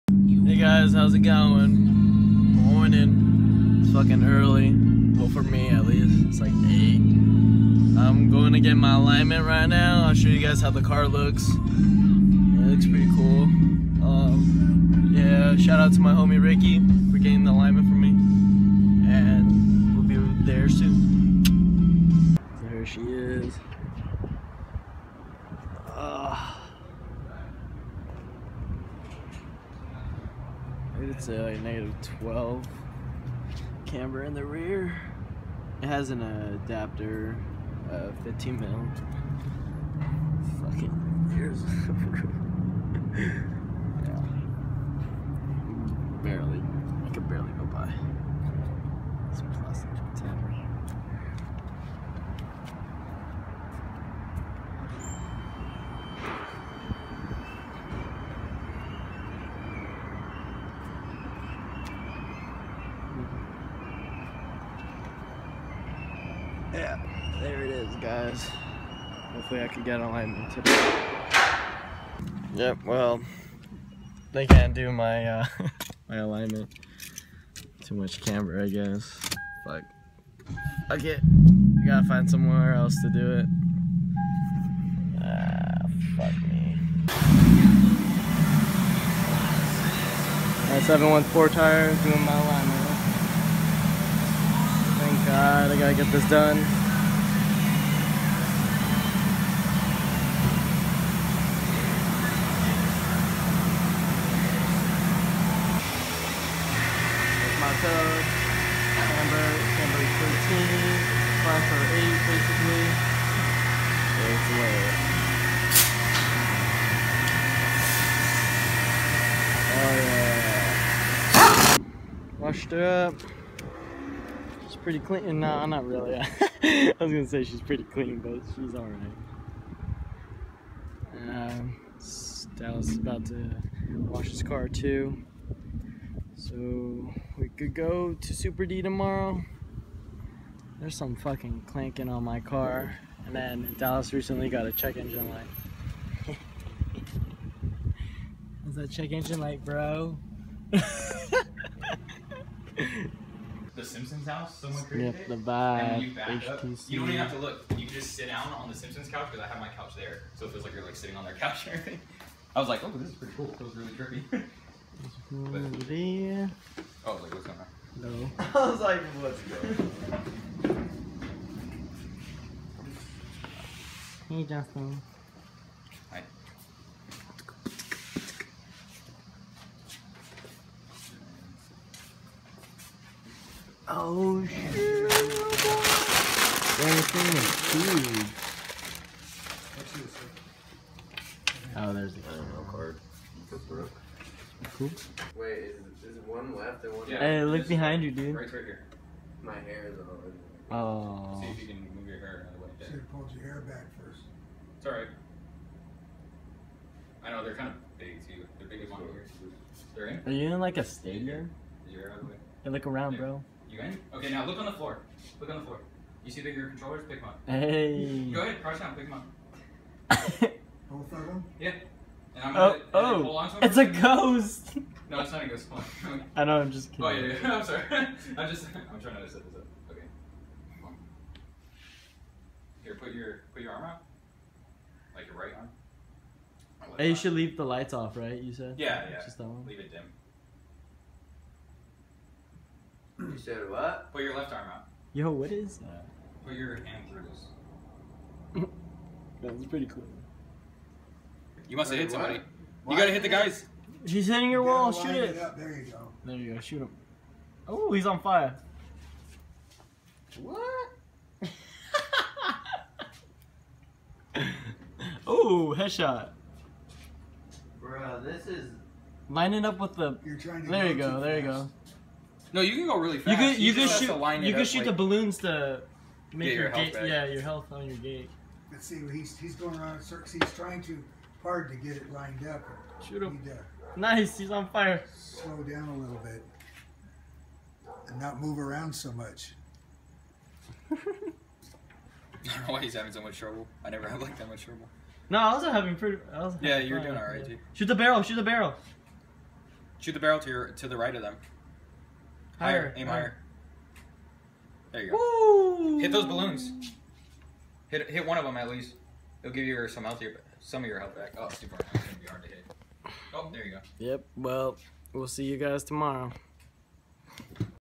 Vlog #5 getting the is aligned and VIVE!
Getting the is300 finally aligned and also played the vive at the end!